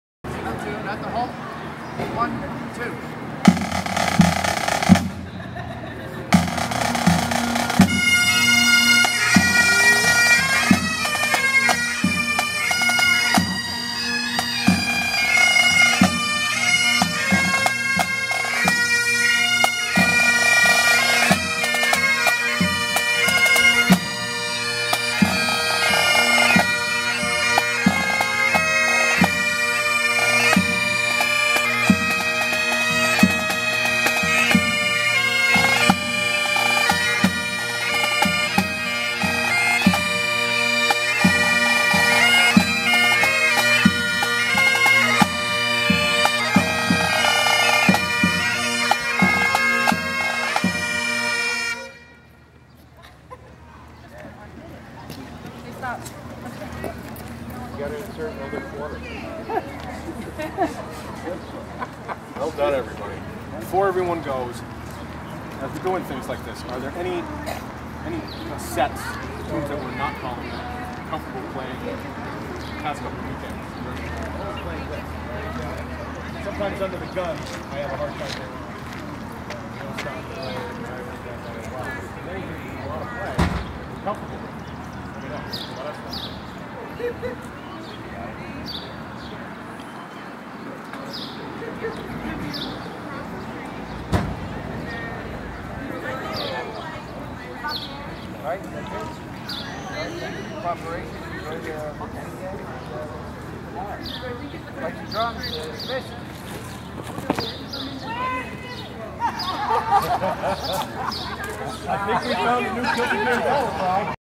Springfield Kiltie Band ~ Mount Holyoke College ~ May 2014
The Springfield Kiltie Band performed at Mount Holyoke College on May 24th, 2014. The occasion was the alumni reunion. Here they are performing ____________ next to Mary E. Woolley Hall.